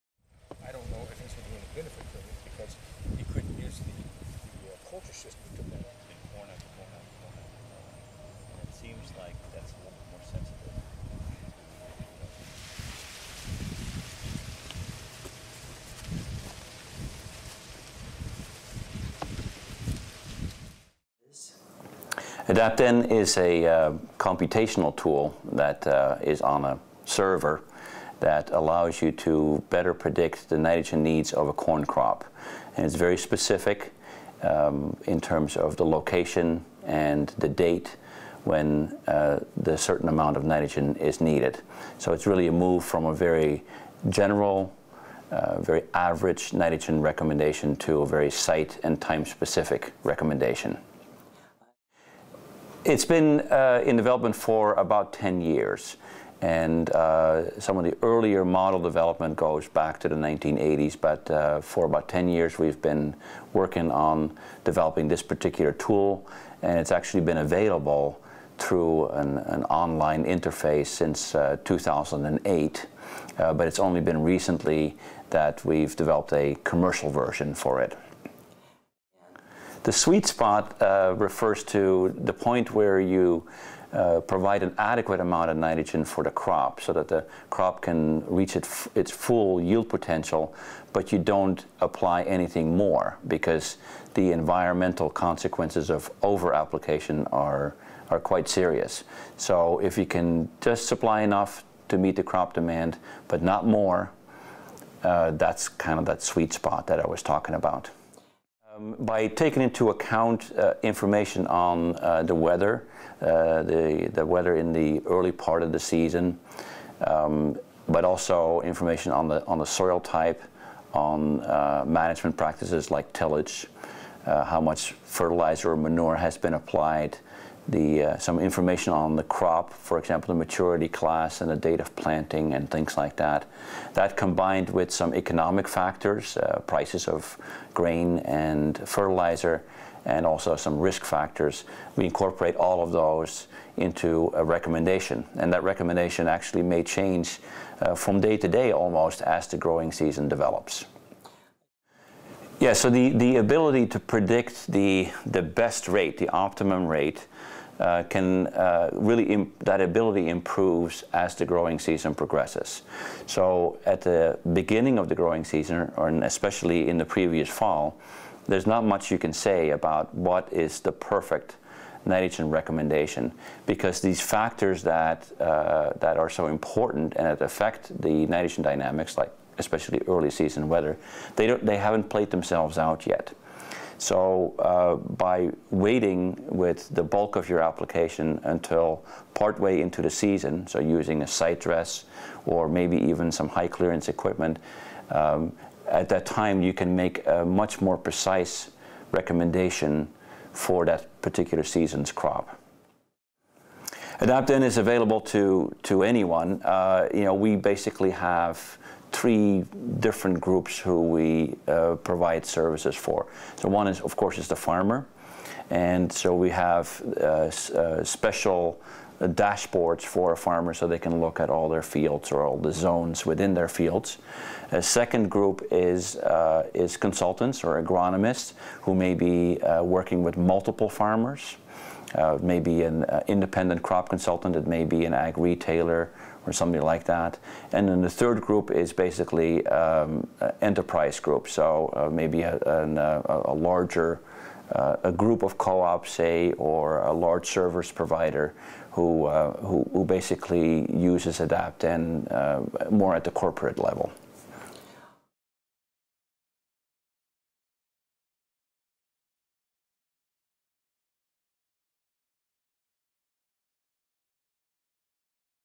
Adapt-N: Hitting the Nitrogen Sweet Spot, with Dr. Harold van Es 1
Your questions about Adapt-N answered in a 3-part video.